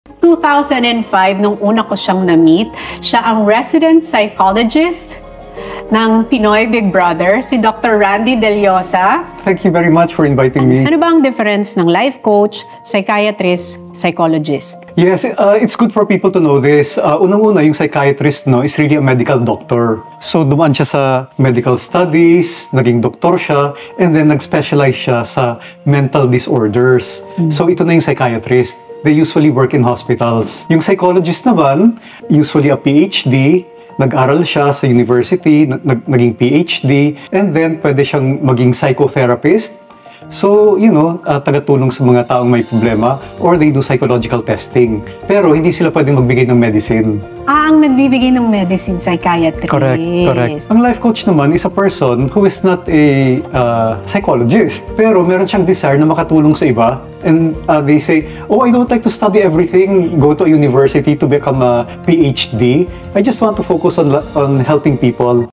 Difference between Psychologist, Psychiatrist and a Life Coach
Dr. Randy Dellosa explains the difference between Psychologist, Psychiatrist, and a Life Coach.

*A Psychiatrist is a medical doctor who specializes in mental disorders.

*A Psychologist also called Psychotherapist conducts psychological testings but cannot prescribe medicines.

*A Life Coach is a type of wellness professional who helps people make progress in their lives in order to attain greater fulfillment.